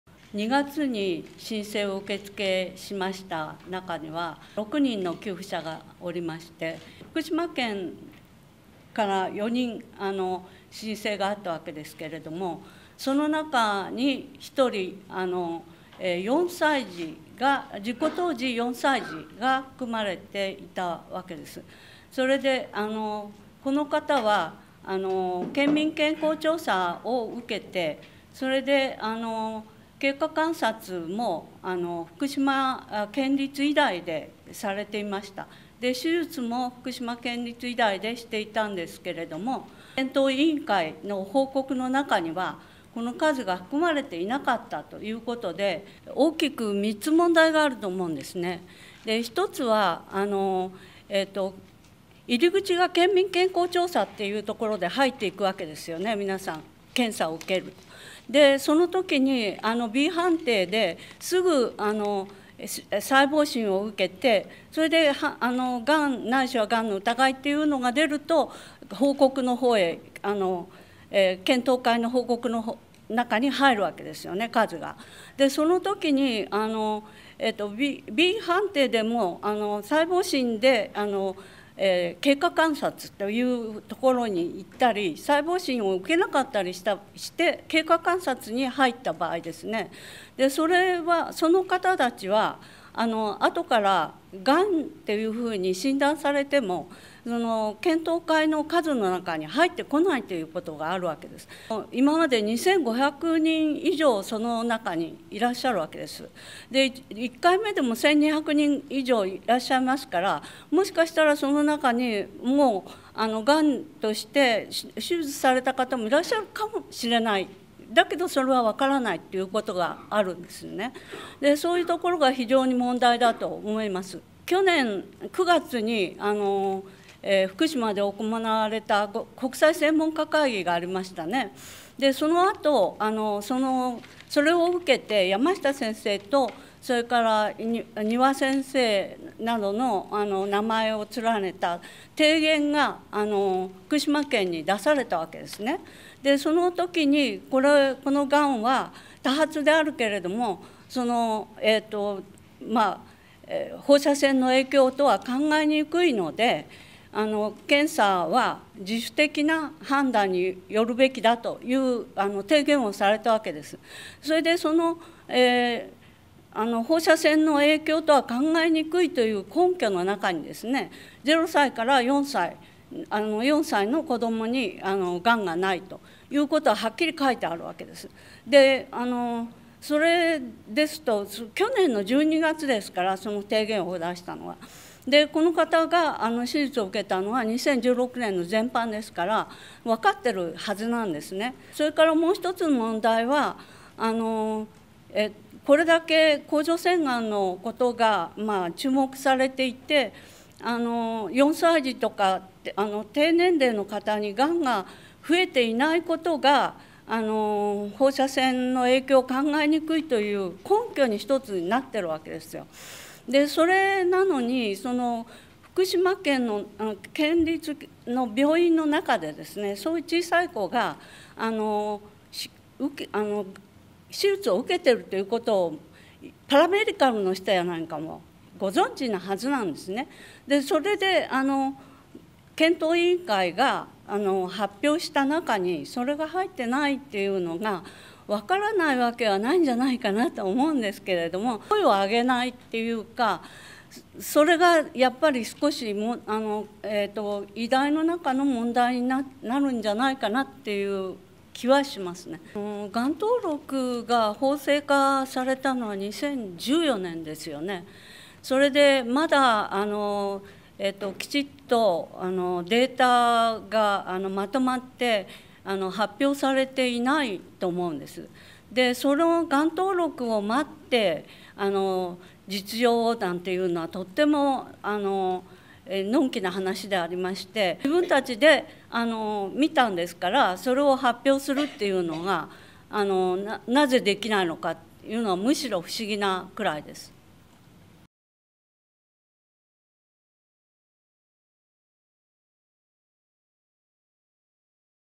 未公表の４歳児へ給付〜甲状腺がん子ども基金
甲状腺がんの子どもを支援しているNPO法人「３・１１甲状腺がん子ども基金」は３１日、福島県の公式データには含まれていない事故当時４歳児に、要領費を給付したと発表した。同基金の崎山比早子代表理事は、「現在、福島県で公表されているデータは、福島県で発症している小児甲状腺がんの一部にすぎないと強く認識した。」と批判している。